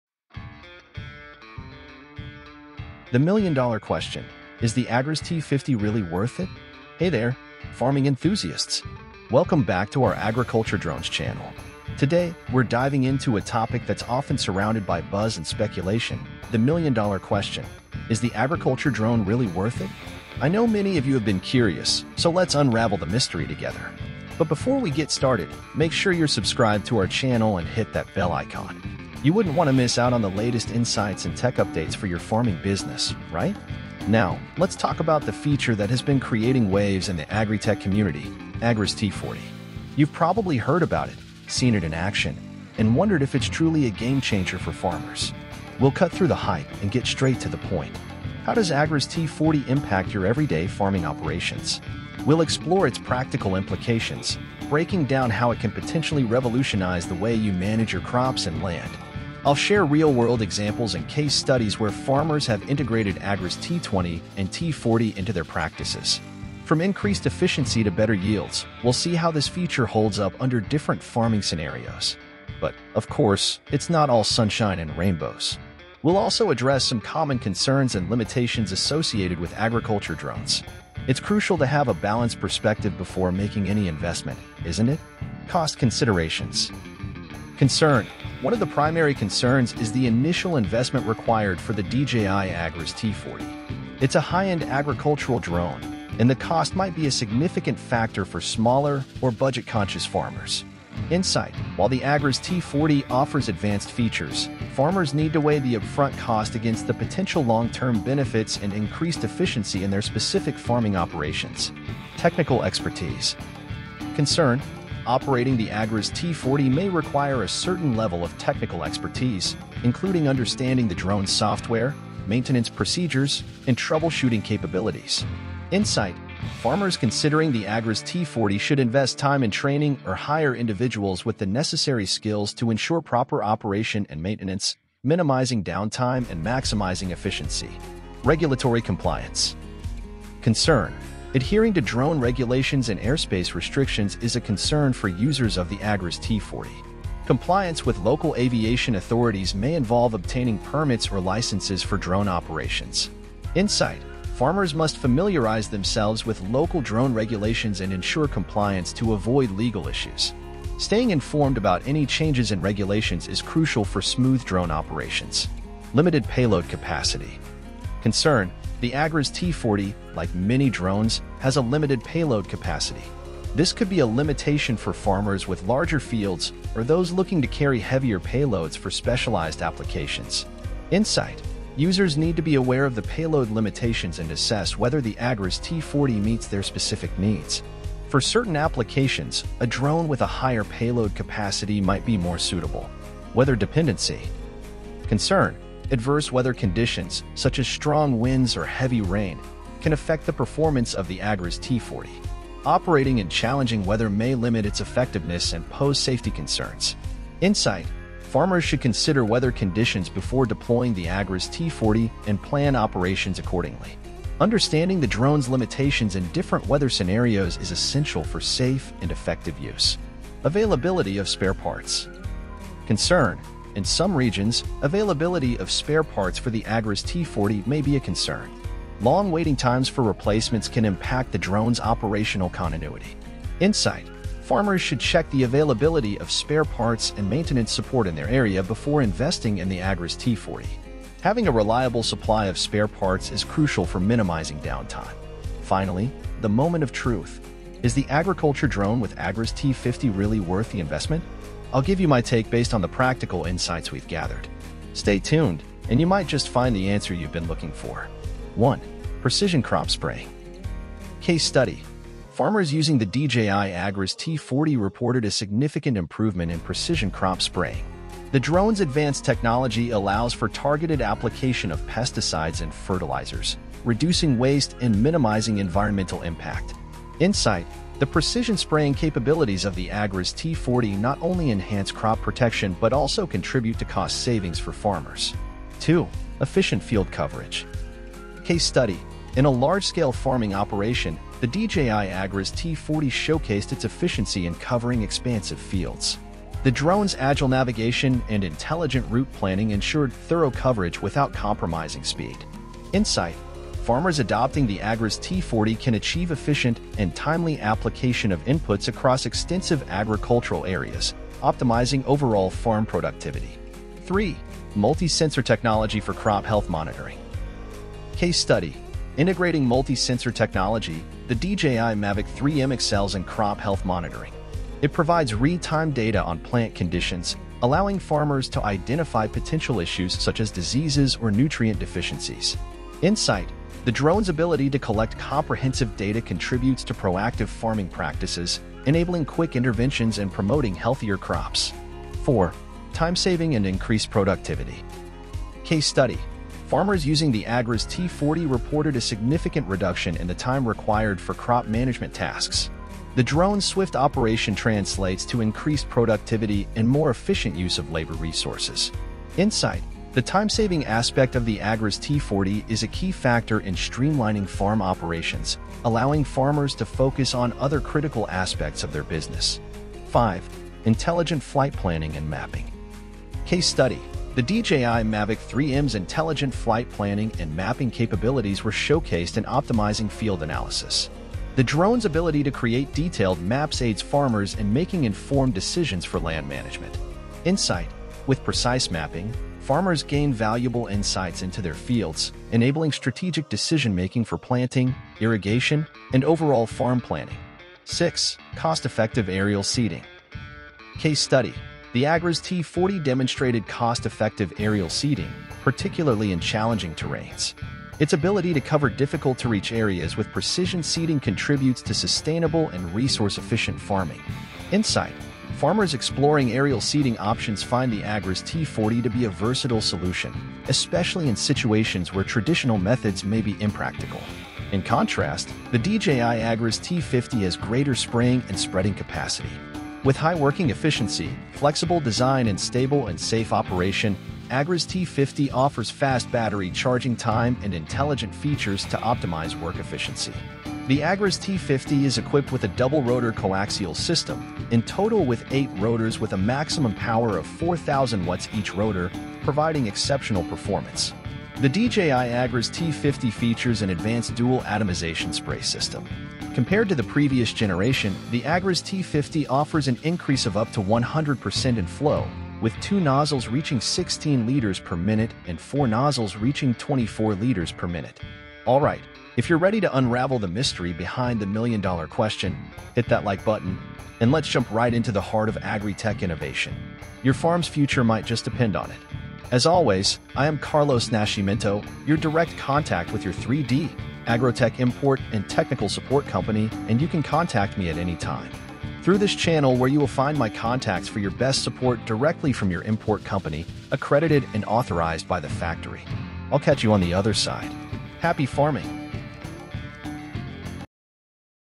The Million Dollar Question: Unraveling the Drone Revolution in Agriculture
🤔💰 Curious about whether Agriculture Drones are Truly Worth It? 🌱🚁

Join us as we delve into "The Million-Dollar Question" in our latest YouTube video! Explore a specific feature in drone technology that's often hyped, and let's discuss its practical implications and whether it's genuinely worth the investment for farmers.

🌿💡 Uncover Practical Insights:
We'll dive deep into the features and capabilities of the DJI Agras T40 drone through summarized case studies and insightful discussions. Discover how this cutting-edge technology is reshaping the landscape of modern agriculture.

🛠️🔍 Addressing Concerns and Limitations:
We'll also address common concerns and limitations associated with the DJI Agras T40, providing a balanced perspective on its usage and effectiveness in real-world farming scenarios.

👨‍🌾📈 Empowering Farmers:
Learn how this innovative drone is empowering farmers with enhanced efficiency, precision, and sustainability, revolutionizing the way they approach crop management and resource allocation.

Whether you're a seasoned farmer or simply curious about the intersection of technology and agriculture, this video is packed with valuable insights you won't want to miss! 🌾✨

Introduction and Presentation: 0:00 - 0:45 (45 seconds)
Overview of Agriculture Drones: 0:46 - 1:30 (44 seconds)
Exploring DJI Agras T40 Features: 1:31 - 3:00 (1 minute and 29 seconds)
Summarized Case Studies: 3:01 - 5:00 (1 minute and 59 seconds)
Addressing Concerns and Limitations: 5:01 - 6:30 (1 minute and 29 seconds)
Insights and Practical Applications: 6:31 - 7:30 (59 seconds)
Conclusion and Call to Action: 7:31 - 8:21 (50 seconds)